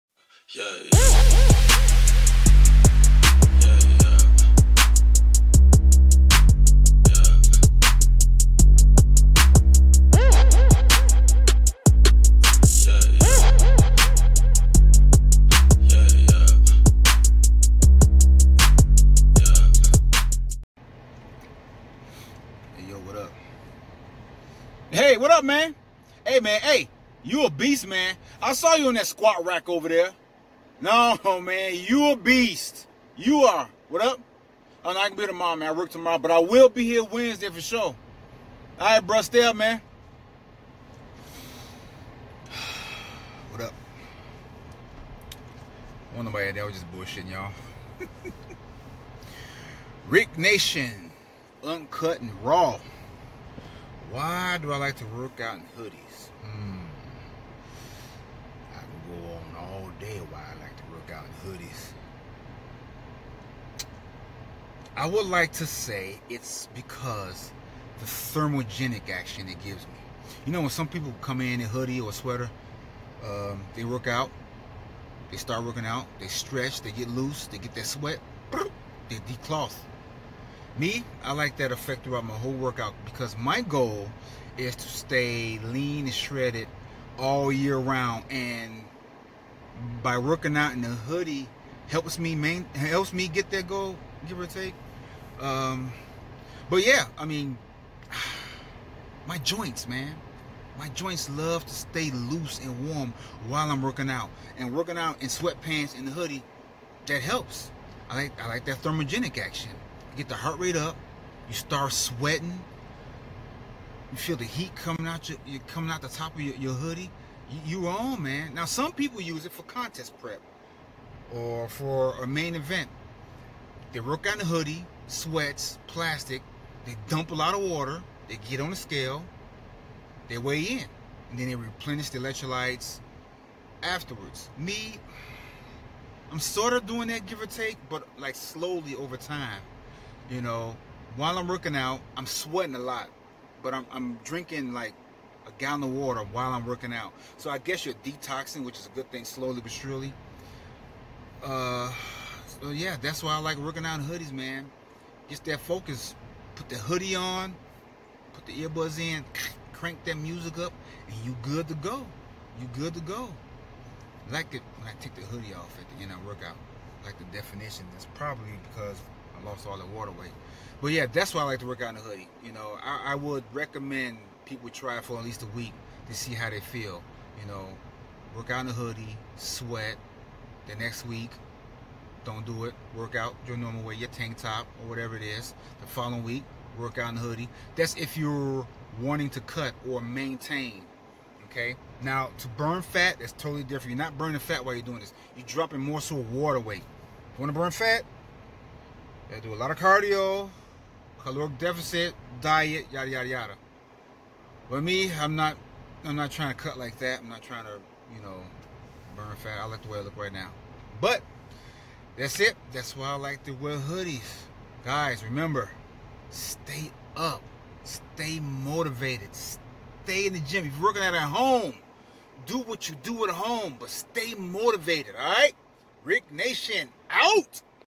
Hoodie Benefits | Can Hoodies Be Beneficial?
How is everyone. First i'd like to thank everyone for stopping by and supporting me. Means the world! Now... Welcome to my FIRST Q&A video. Hoodies. Why do i wear them? Do hoodies help with fat loss. Can hoodies be beneficial? These are all great questions that i answer here.  Thanks again for dropping by and supporting me.